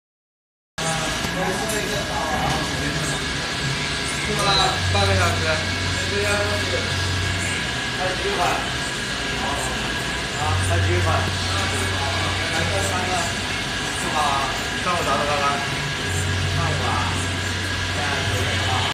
How does Inkjet Photo Printer machine work before sublimation custom ice hockey jerseys
How does inkjet photo printer machine work befroe sublimation custome ice hockey jerseys? If you'd like to find out this answer, watch this video, you are will be clear! For more info. ,why not have more try to this thanks!